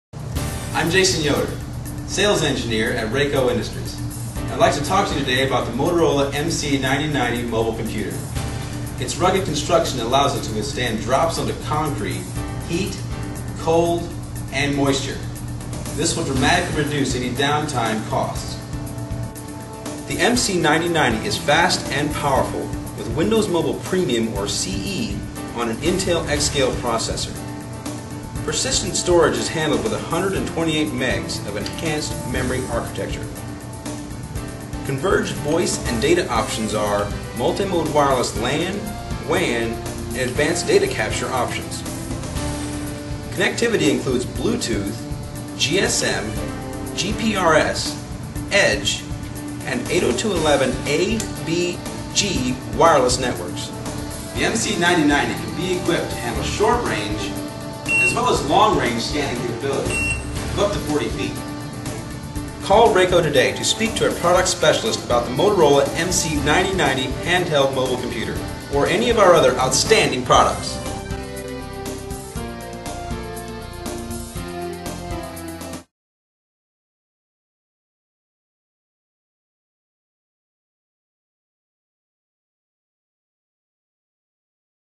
Motorola MC9090 Video Presentation
Introduction to the Motorola/Symbol MC9090 mobile computer w/ integrated barcode scanner